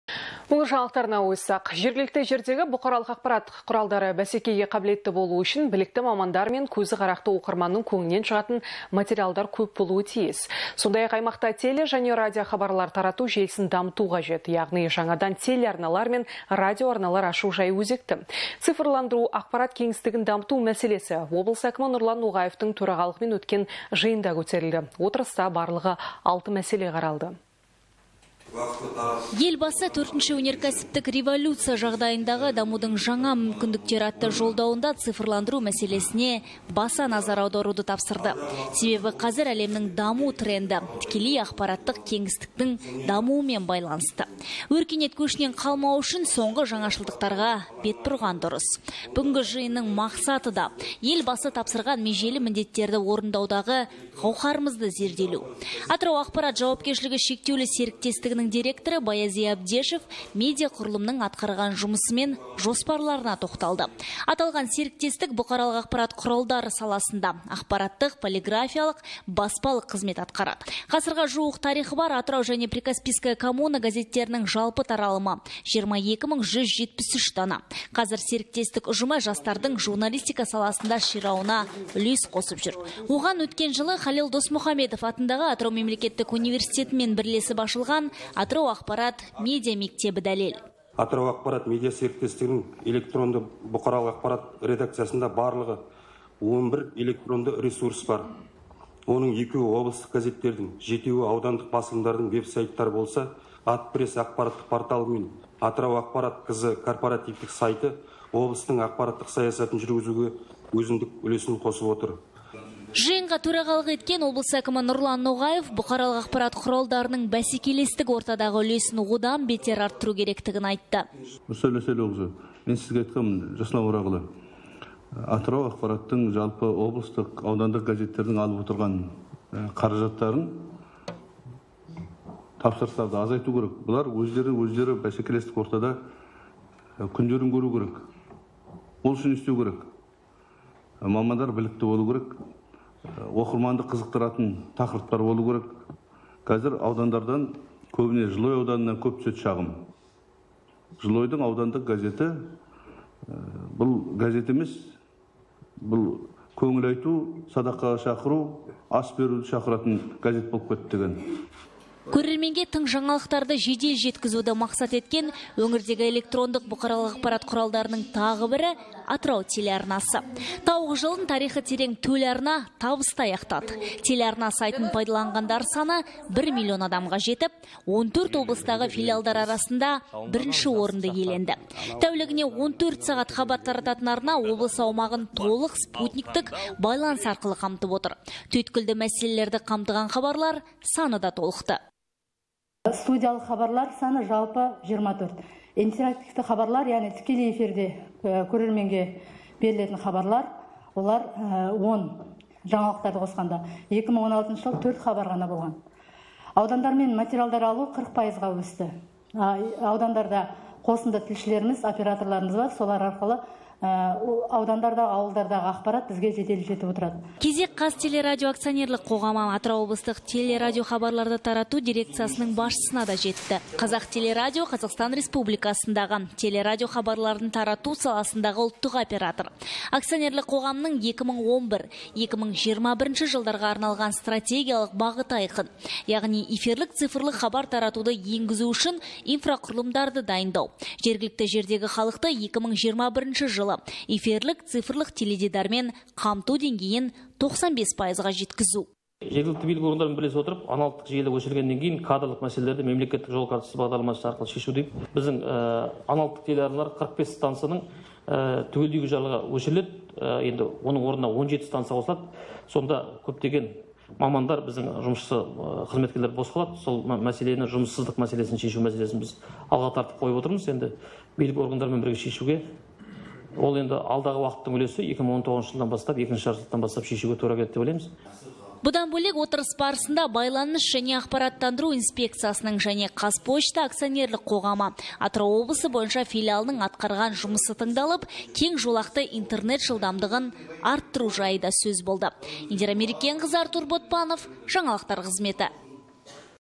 Байланыс және ақпарат саласындағы мәселелер талқыланды
Жергілікті жердегі БАҚ бәсекеге қабілетті болу үшін, білікті мамандар   мен көзі қарақты оқырманның  көңілінен шығатын материалдар  көп болуы тиіс.Сондай-ақ,  аймақта теле және радио хабарлар тарату желісін дамыту қажет. Яғни,  жаңадан телеарналар мен радиоарналар ашу жайы өзекті. Цифрландыру,ақпарат кеңістігін дамыту мәселесі облыс әкімі Нұрлан Ноғаевтың төрағалығымен өткен жиында көтерілді. Отырыста   барлығы 6 мәселе қаралды